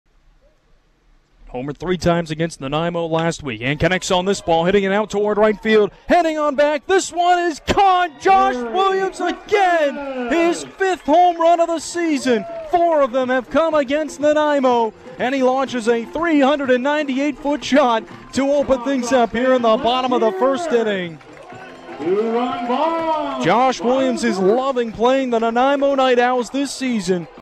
Josh Williams Home Run 7.21.23
Josh Williams hits his fifth home run of the summer to put the AppleSox on the board first and help lead them to a 5-1 win over the Nanaimo NightOwls.